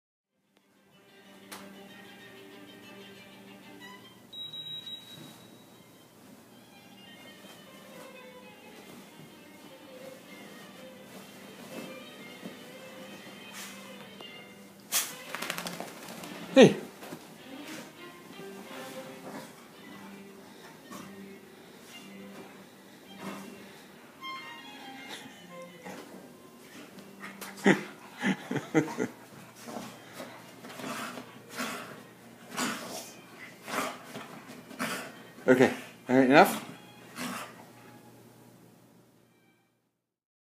Monkey the Terrier Wakes from A Bad Dream
My Wheaten, Monkey Eloise, likes to hide under the sheets after a bath.  She also likes to go crazy.